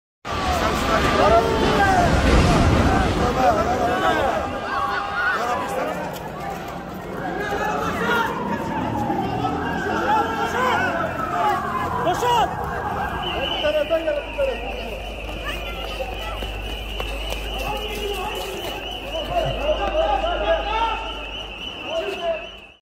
7.8-Magnitude Earthquake Hits Turkey & Syria, Leaving 2,300+ Dead
A 7.8-magnitude earthquake hit Turkey and Syria Monday, leaving more than 2,300 people dead and thousands injured. It's reportedly the strongest quake to hit the region in over 100 years.